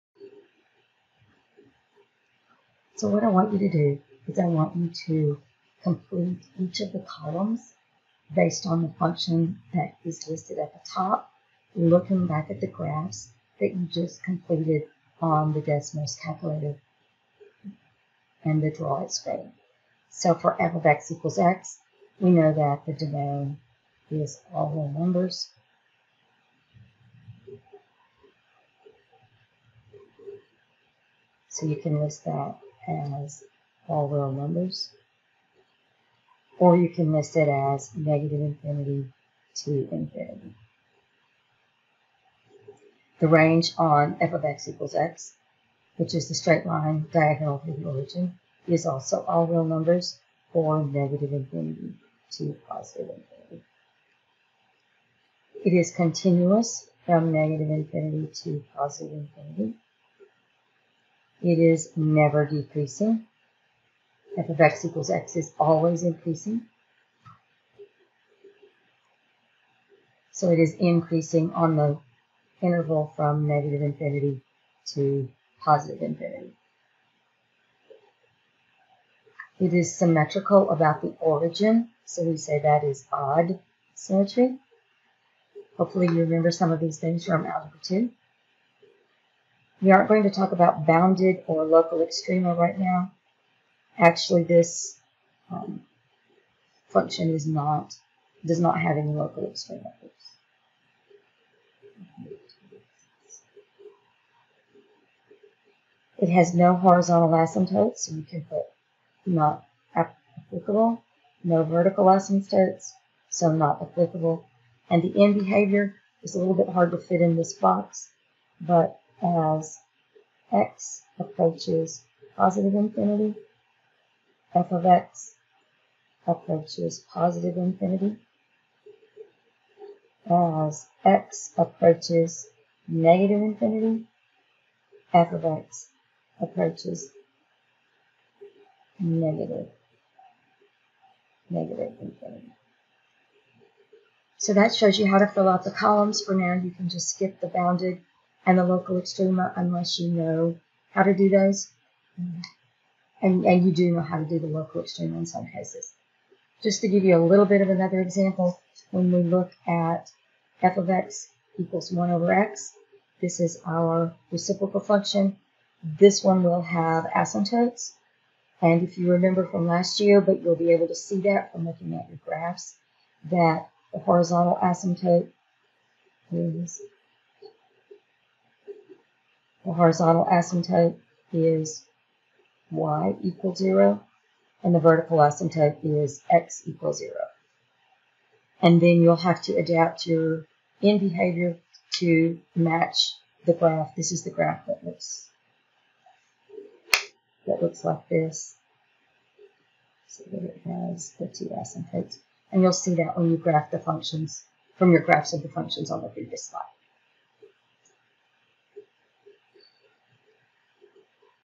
Parent Function Chart Instructions
Directions to students on how to complete their parent functions chart